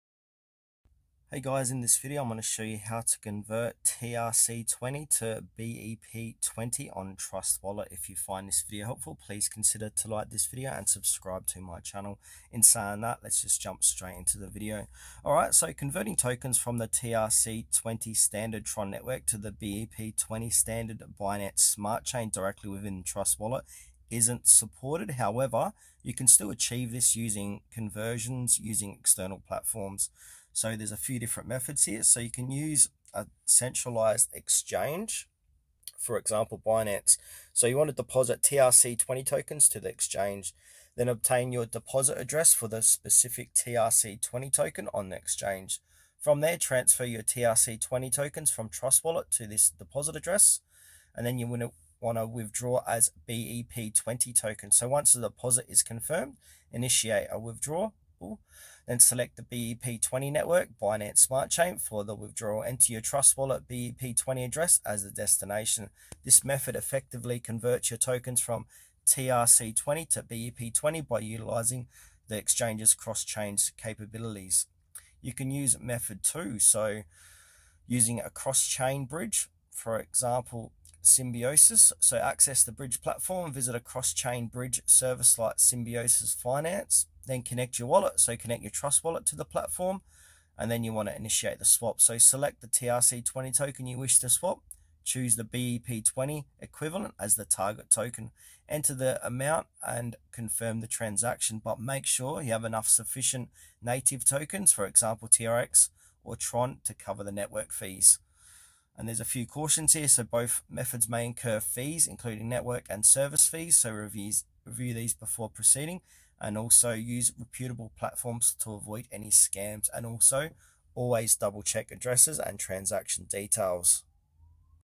How To Convert TRC20 To BEP20 On Trust Wallet
In this tutorial, we will show you how to convert TRC20 to BEP20 on Trust Wallet. Follow these simple steps to easily convert your tokens and move them to the Binance Smart Chain network!